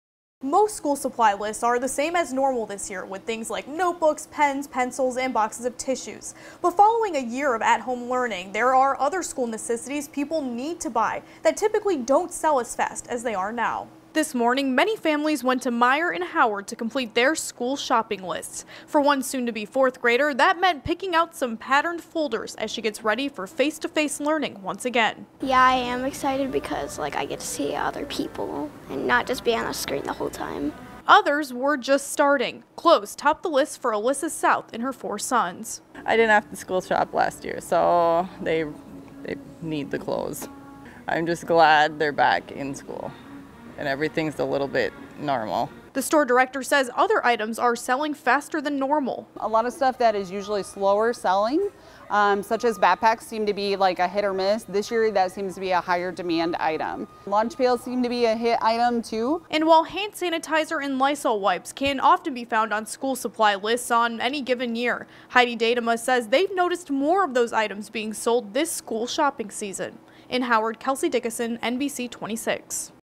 Families go back-to-school shopping before in-person learning resumes
A return to in-person learning means a return to the store for back-to-school supplies.